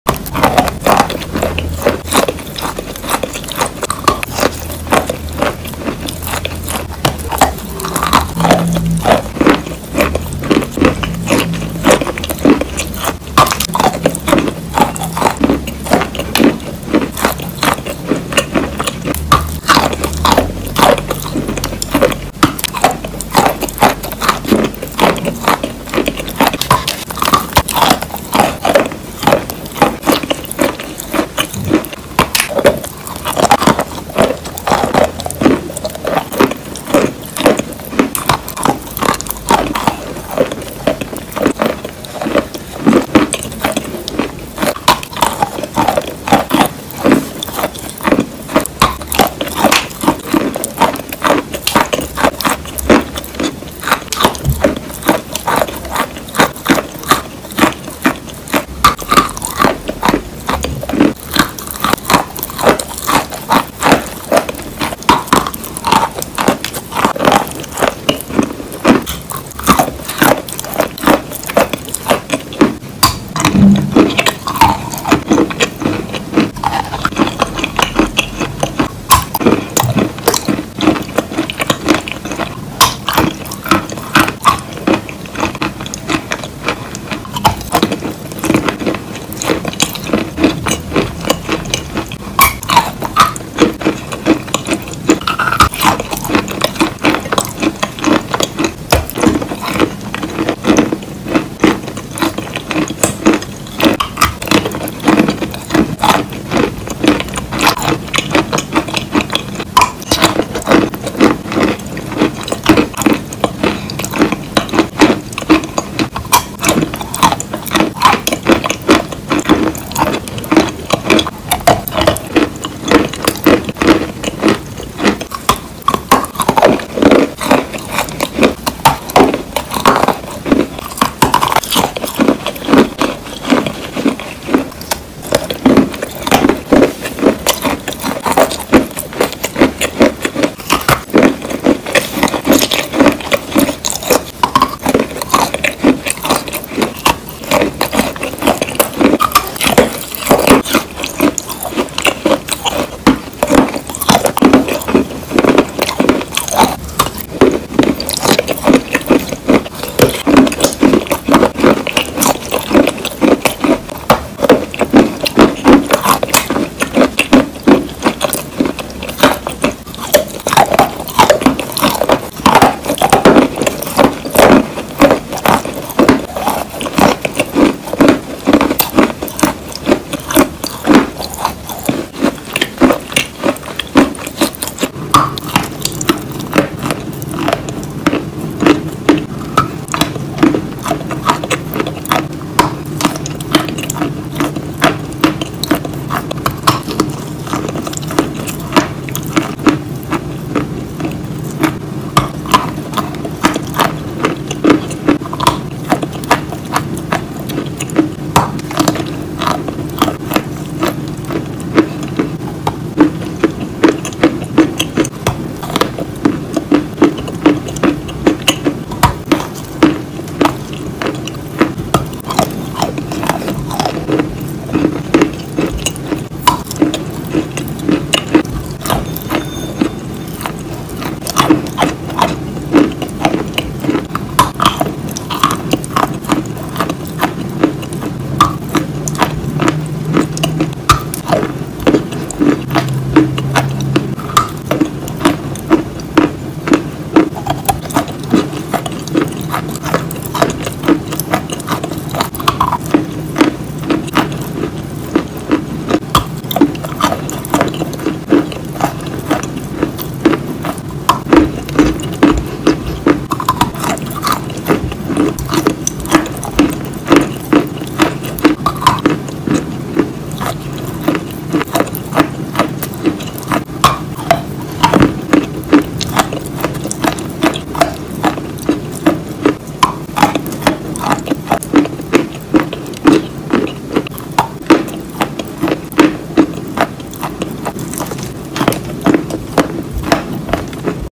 Completion crunchy asmr relaxing
Dry and  wet chalk crunch with creamy paste